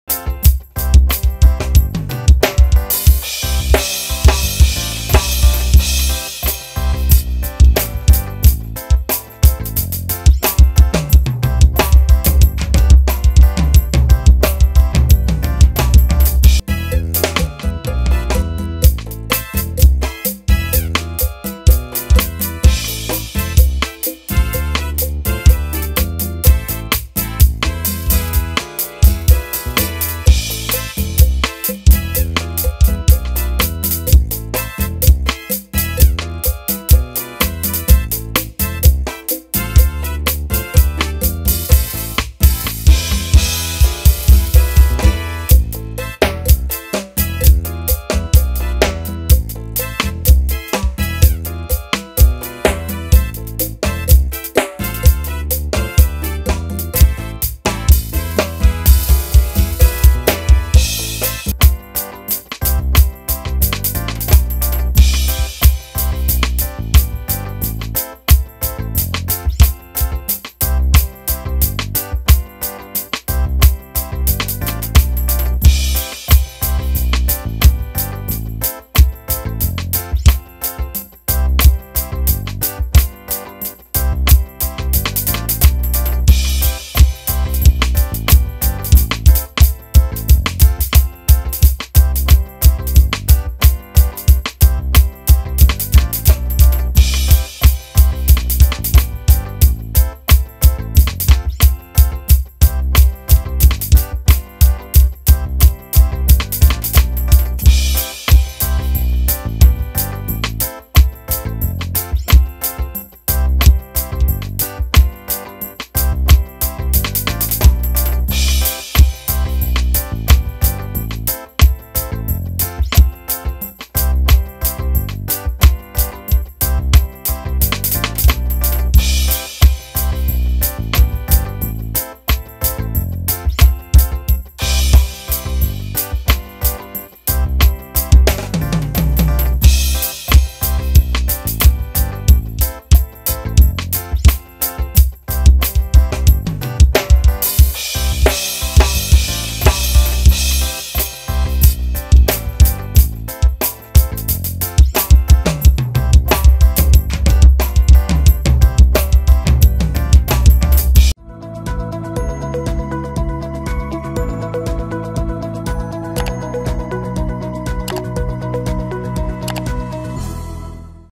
Groove Time With 9 year old Drummer
🥁 Meet Denzel, the 9-year-old drum prodigy who's setting the rhythm ablaze! 🌟 Watch in awe as he unleashes his Sunday groove on the drums, showcasing his incredible talent and passion for music. 🎶 Denzel isn't just your average drummer - he's a multi-instrumentalist with a love for all kinds of music. 🎸🎹 Join him on his musical journey as he drums his way towards a brighter future! Don't miss out on this young sensation's electrifying performance! 🔥